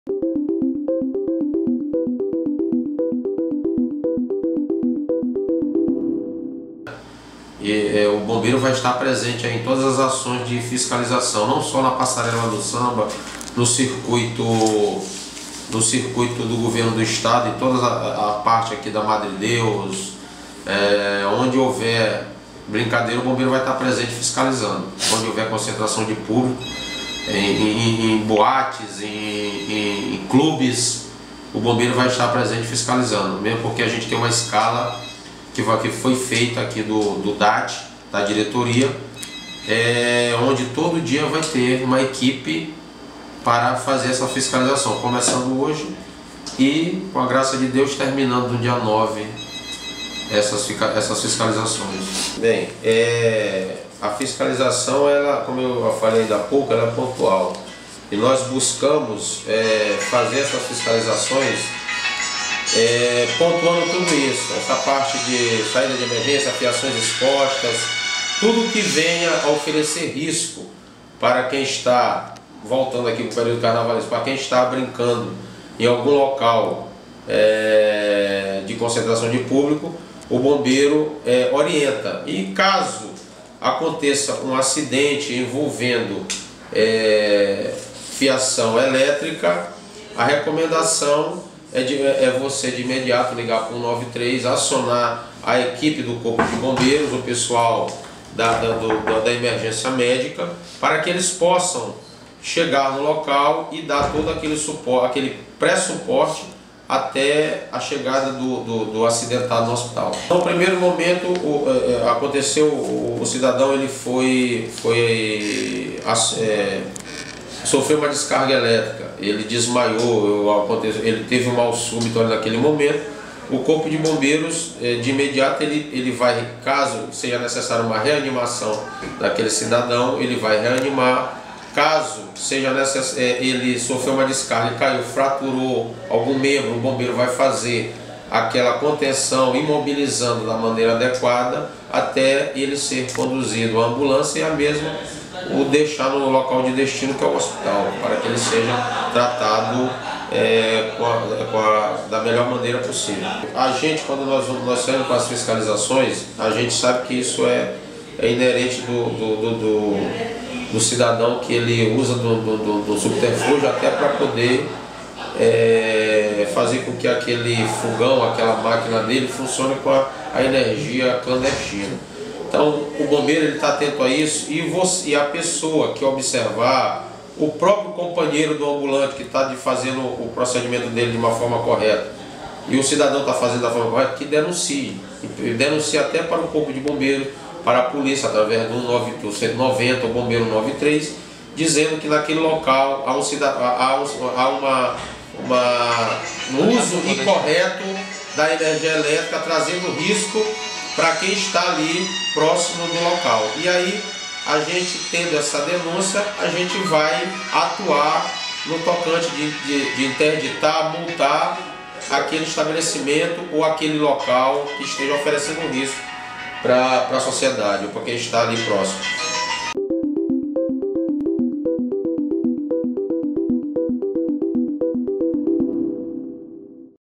Corpo de Bombeiros reforçará fiscalização durante Carnaval
Atividades serão desenvolvidas em todos os locais de aglomeração de público durante as festas para evitar acidentes, como na Passarela do Samba&nbsp;